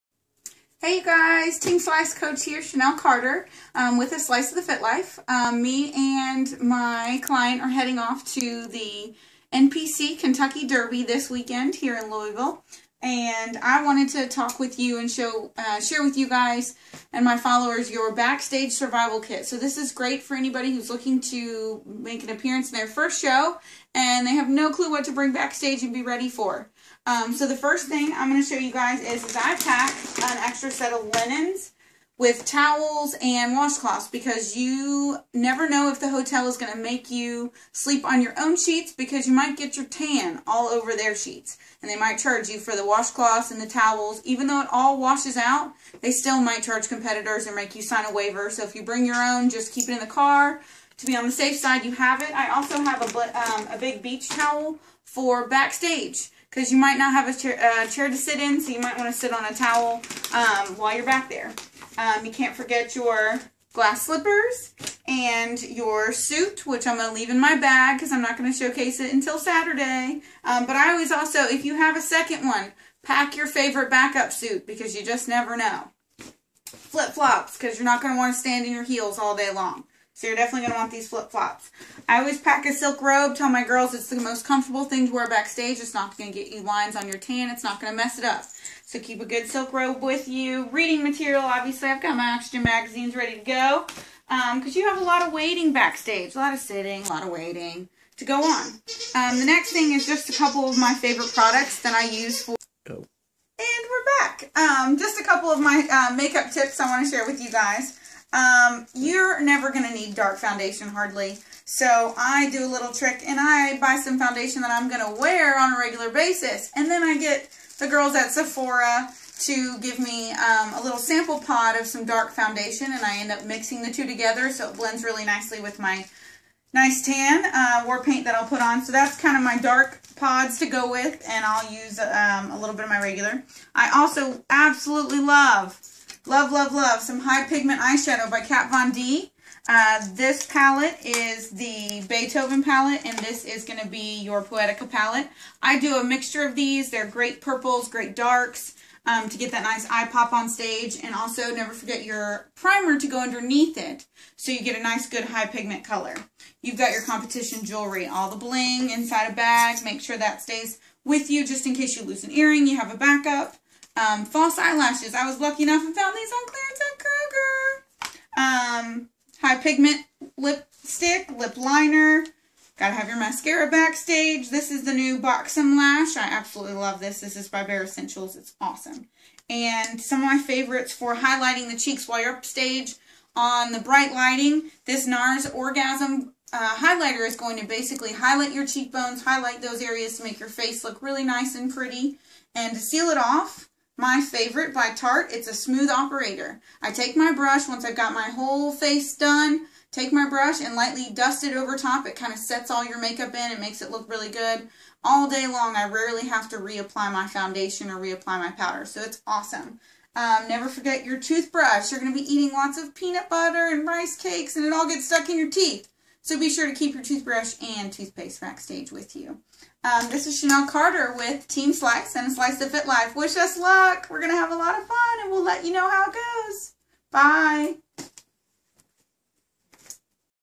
Team Slice Survival Kit
For physique competitors (bikini, figure, npc, fitness america etc) looking to tackle their first show and you don't know what to bring back stage with you!